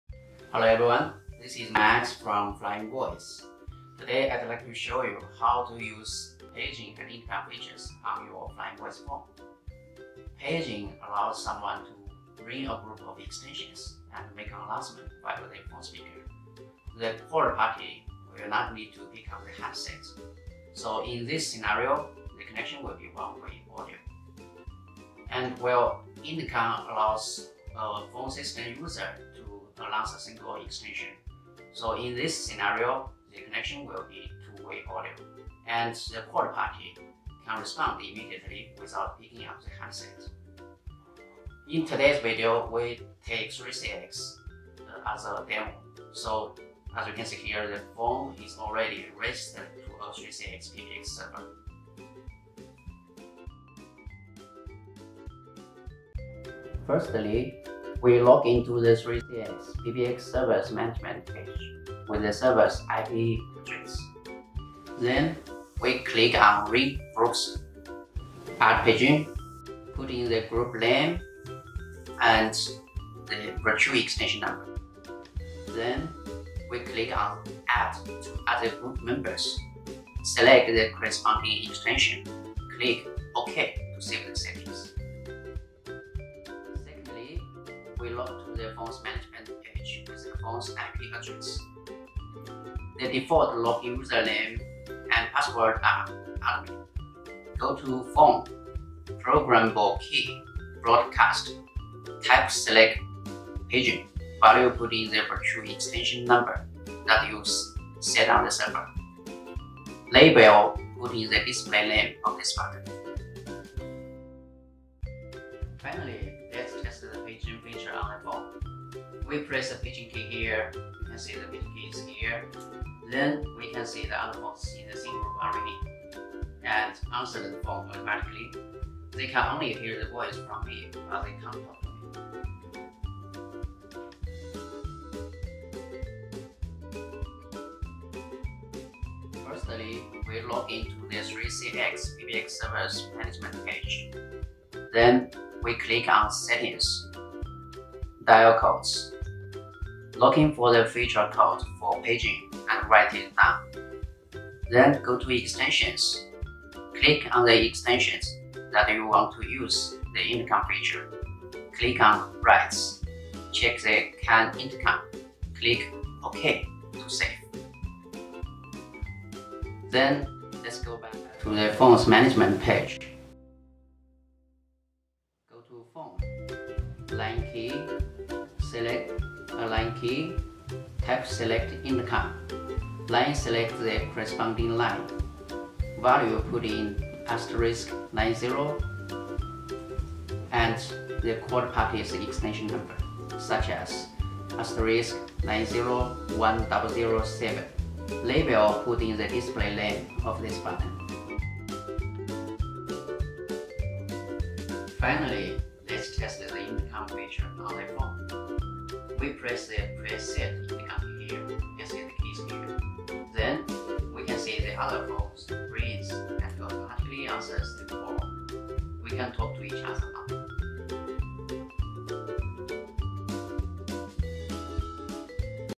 How to Use Paging and Intercom with 3CX PBX and Flyingvoice Phones
The Paging and Intercom features are widely used in a variety of businesses. The called parties do not need to pick up the handset as the real-time voice messages will be played via the phone speakers, allowing them a faster response or action.
Let’s see how to set it up with 3CX PBX and Flyingvoice Phones: